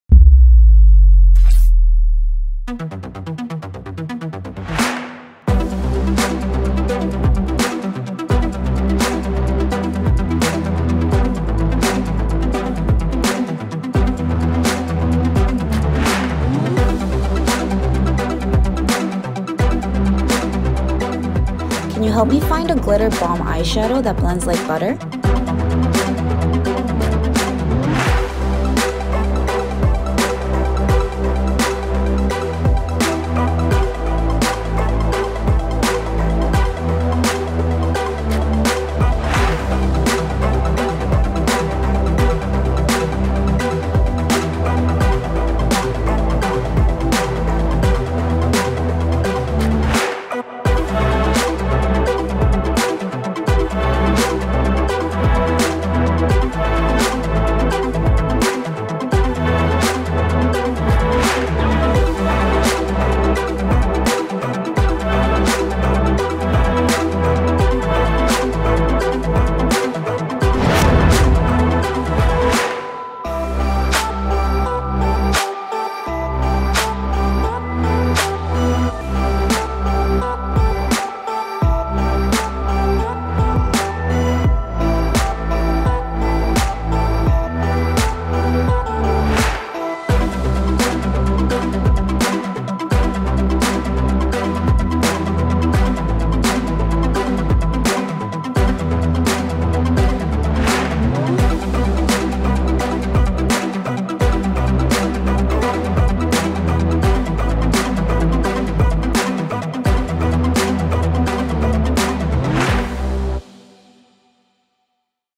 Glamai – Latest Beauty Trends & Reviews with AI
Take out the guesswork in finding your beauty! ​

Glamai is a beauty discovery service that helps you discover the latest trends in skincare, makeup, and other beauty products and matches them with your unique needs and concerns. ​

Find rich, in-depth information about beauty in one convenient place and become a beauty shopper with taste. ​

Powered by artificial intelligence (AI), Glamai provides over 10,400 beauty tastes based on what ‘people actually say’ such as: ​

- Matte lipstick that is perfect for ‘surviving a pizza’ ​
- “camera ready” foundation ​
- Eyeshadow that will give you a ‘galaxy look’ ​
- Easily applied moisturizer ‘For morning rush’ ​

5 features you’ll LOVE about Glamai: ​
- Just pick or say from 10,400 real beauty tastes for search & recommendations ​
- Discover trendy & fun rankings such as “Viral TikTok Masks for Acne” ​
- Compare beauty influencer reviews and what people are actually saying to get real reviews and tips ​
- Update your profile to receive more than 100 million personalized AI recommendations just for you ​
- Check out live-updated AI information on product ingredients, colors, key functions, and more ​

Discover real beauty tastes from people like you! ​